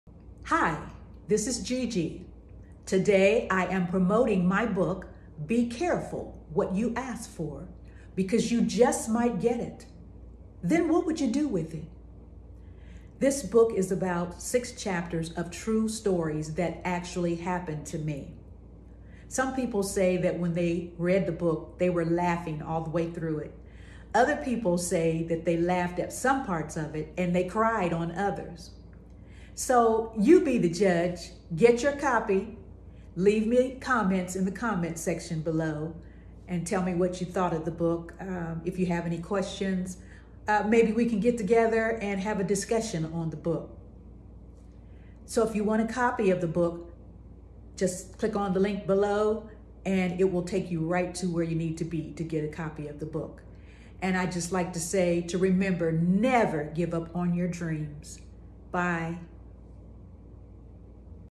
The Vision Trailer # 6
What would you do if this happened to you? Would you stay or would you run? After reading my book, co-workers, friends and others say that they laughed when they read the book. Some say that they shed a few tears.